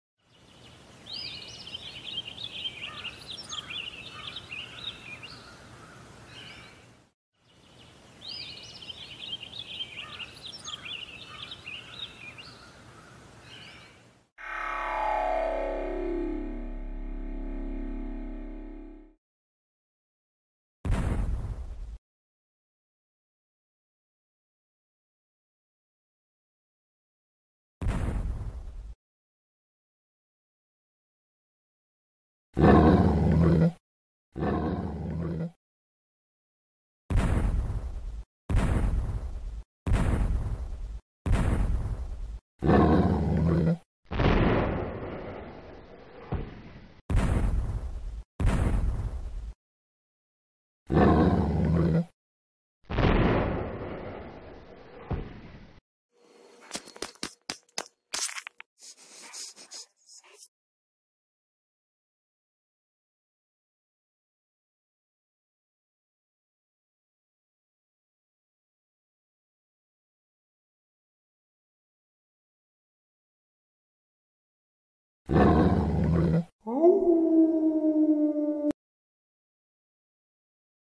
TITOS TERROR!
Tito a giant monster dog wakes up a beast, who is not happy!